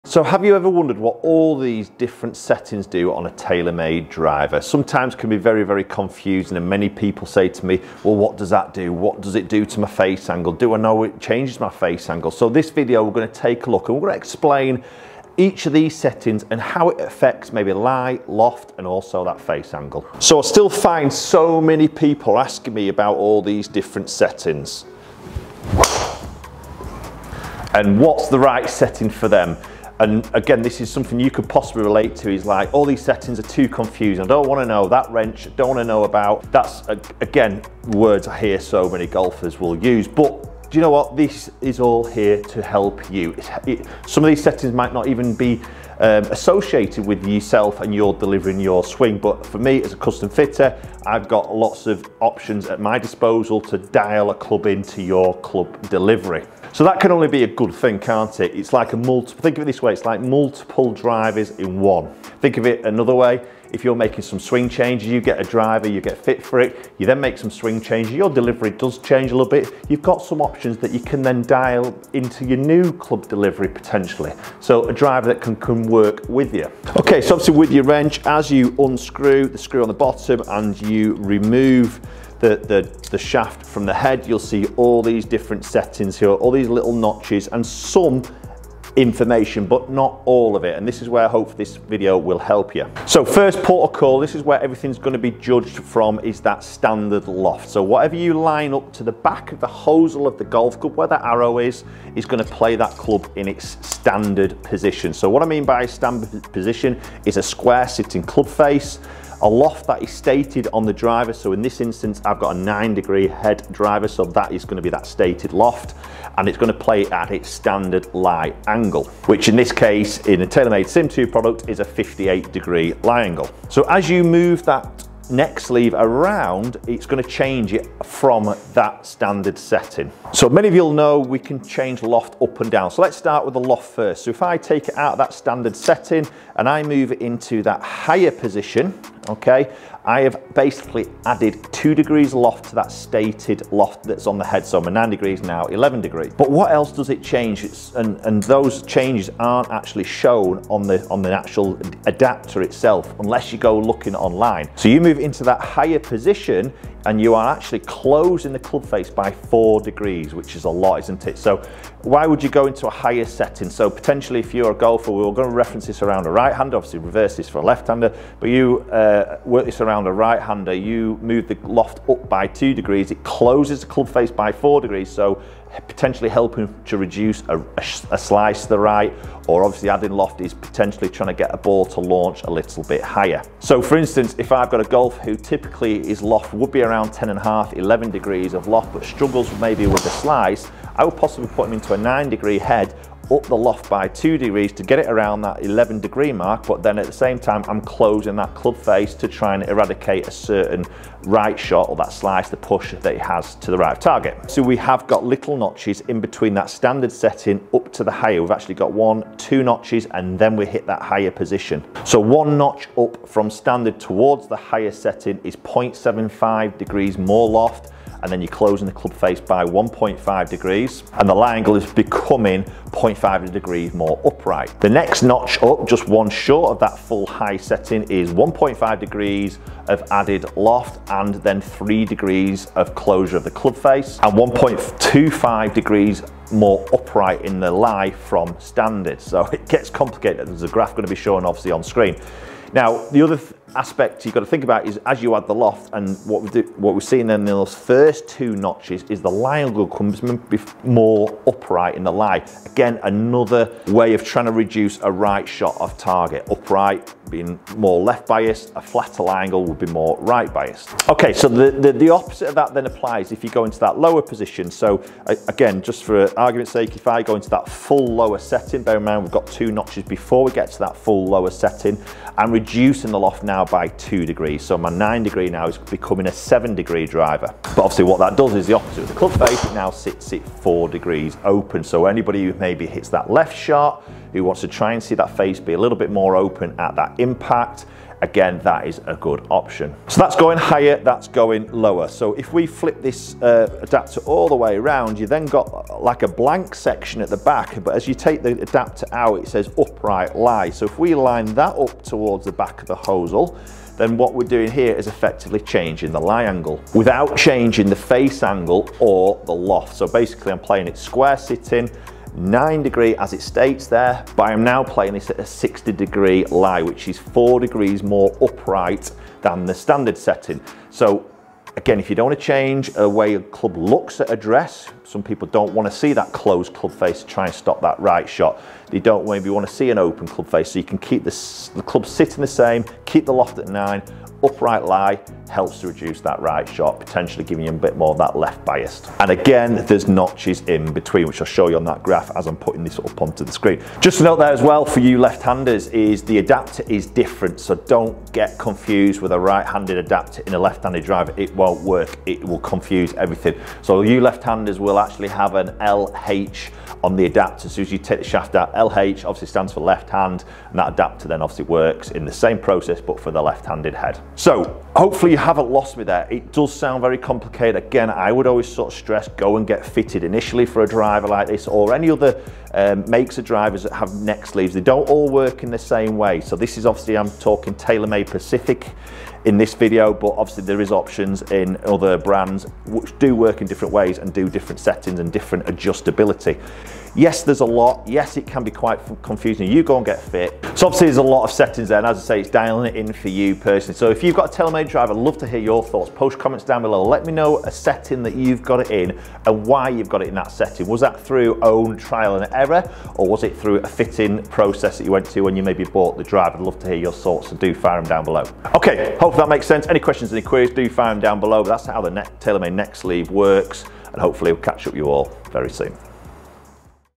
MOST GOLFERS DON'T KNOW THIS & It Can Be Harming Your Golf!!
In this video I explain how the TaylorMade driver adapter neck sleeve works and how it alters loft, face angle & lie angle and how that can affect the golf ball flight and shot direction as well as golf ball curve. A great tool for custom fitting. Can you turn your slice into a draw and hit more fairways and longer tee shots with the driver. this could help you.